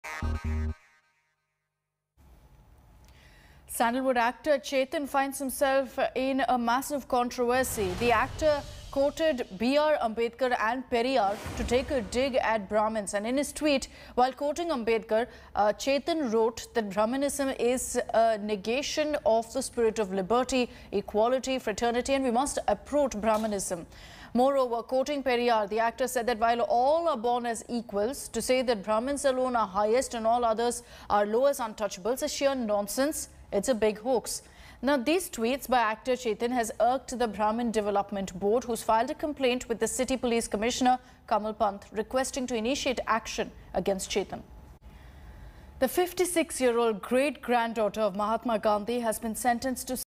Sandalwood Actor Chetan Kumar In Dock Over Quoting Periyar To Take A Dig At Brahmins
A row has now erupted over Kannada actor Chetan Kumar's 'Uproot Brahminism' tweet, where he quotes Periyar to take a dig at Brahmins. A Brahmin body has now filed a complaint against the actor, accusing him of hurting their sentiments and has demanded him to apologise.